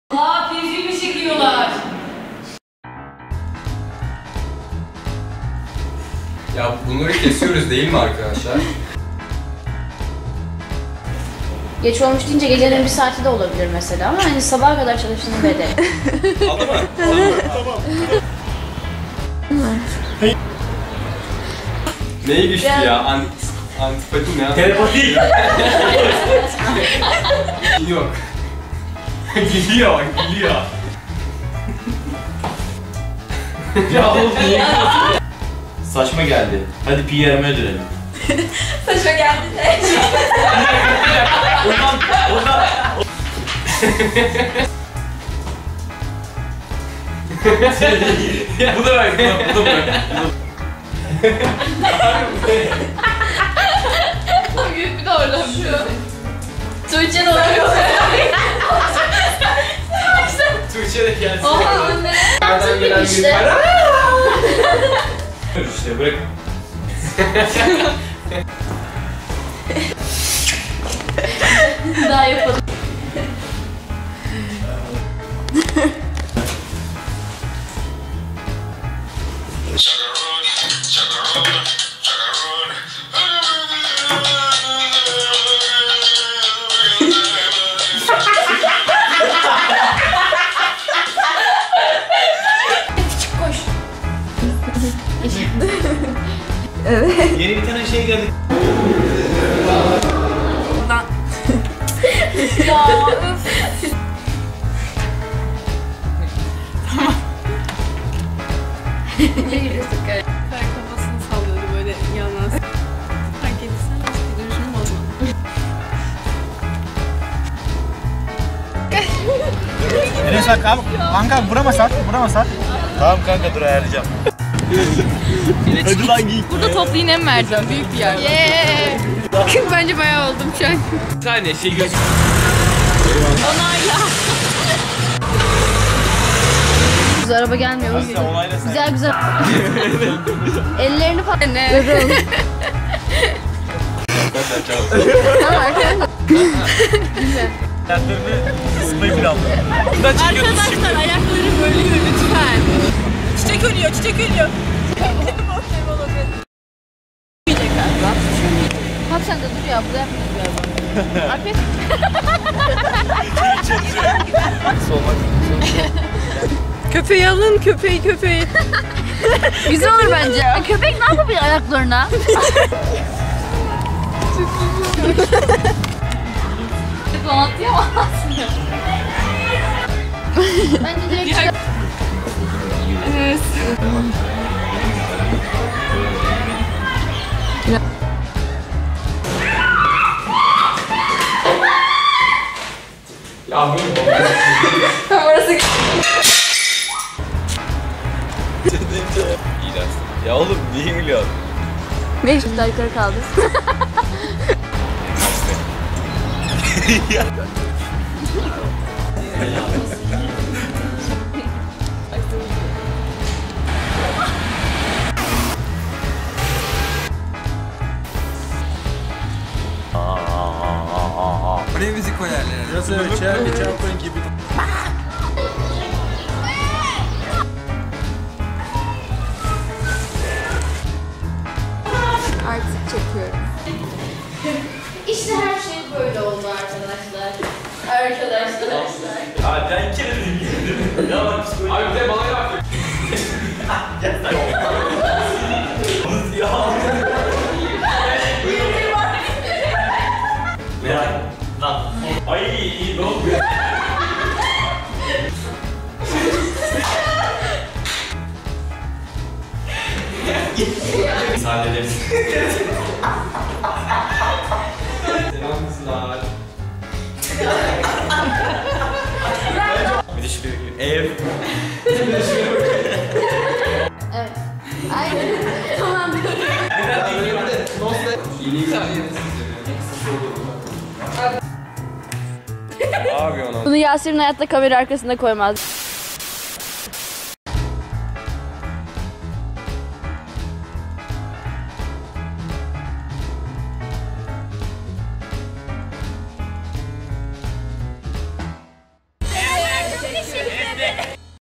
Pilim Adamının İbretlik Hikayesi|Kamera Arkası
İzmir Buca Fen Lisesi 9. Geleneksel Pi Şenliği için çektiğimiz kısa filmimizin kamera arkası. Umarım izlerken keyif alırsınız. Tamamiyle BFL'19A ürünü olan filmimiz içinse aşağıdaki linke tıklayabilirsiniz: